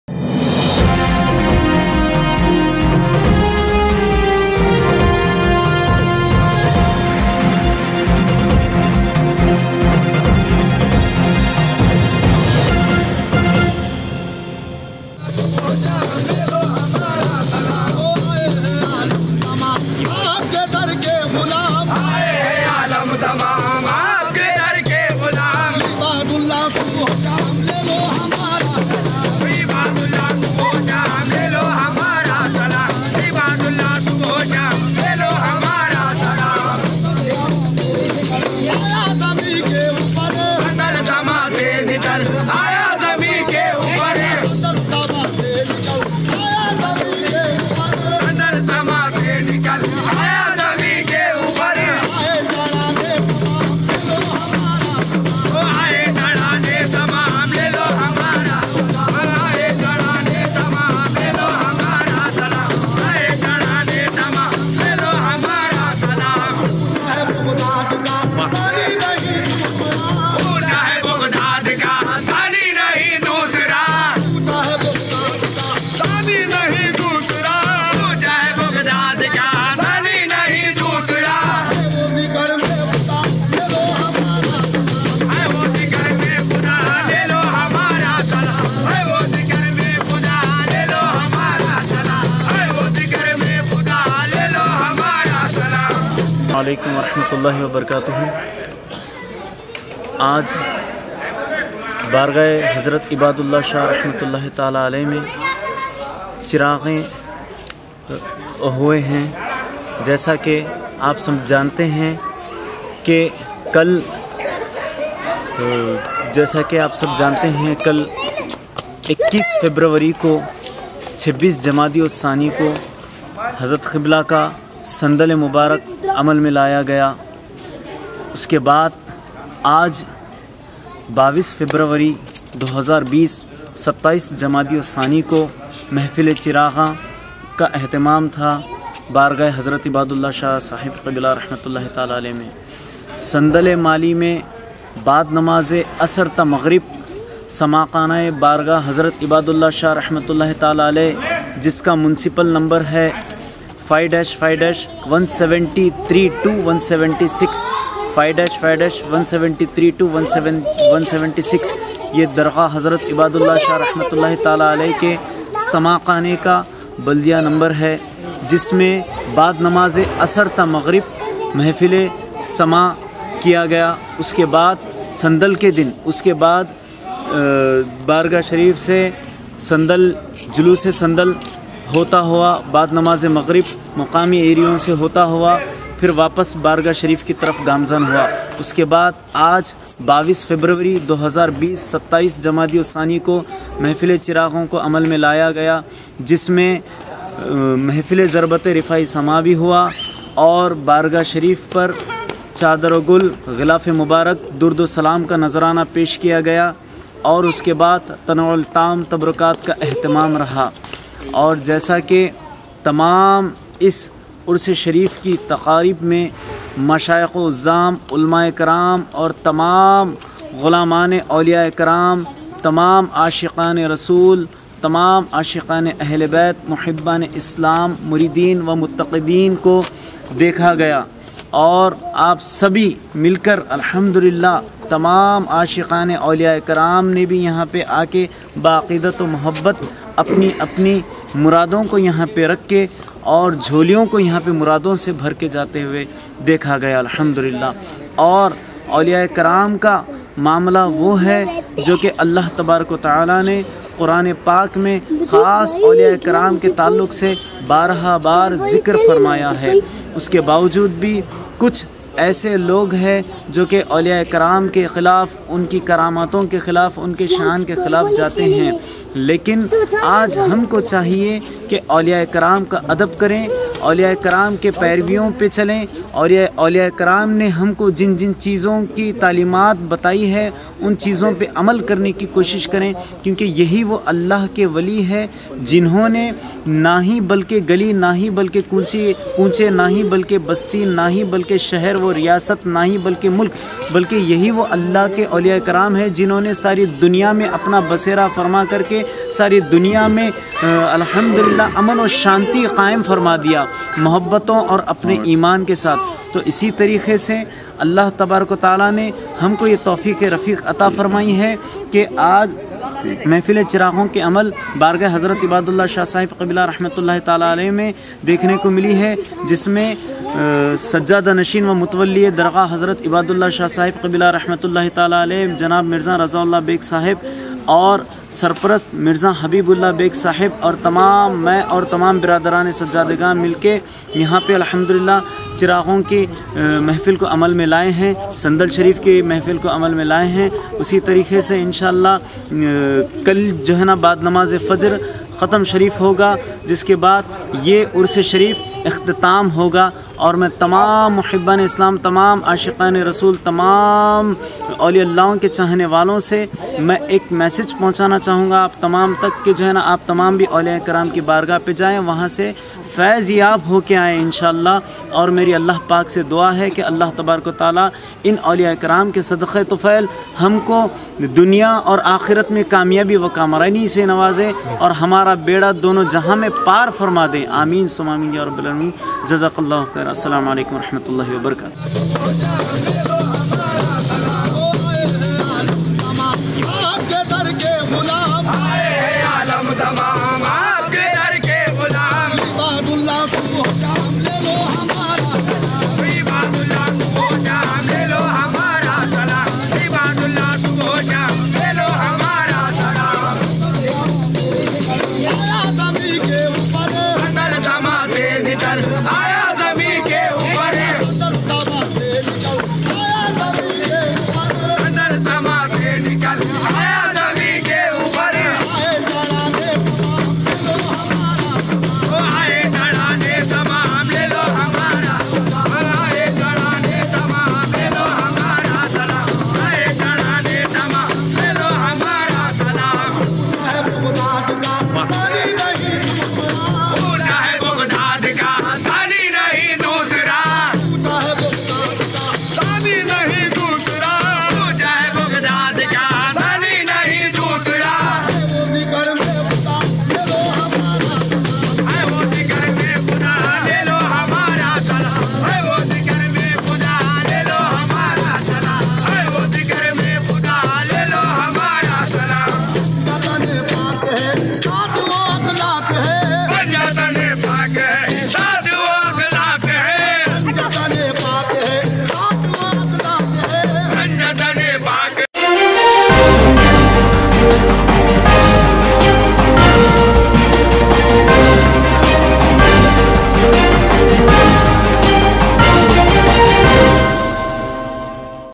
CHIRAGHAAN AFTAB-E-DECCAN HAZRAT IBADULLAH SHAH RH AT GHOUSEPURA DARUSSALAM ROAD NAMPALLY
CHIRAGHAAN ORGANIZER JANAB MIRZA RAZAULLAH BAIG SAHEB AND SAJJAD-E-NASHEEN WO MUTTAWALLI E DARGAH HZT IBADULLAH SHAH SAHEB QIBLA RH,ZERE SARPARASTI JANAB MIRZA HABEEB ULLAH BAIG SAHEB AND ALL BROTHERS OF SAJJADA-E-NASHEEN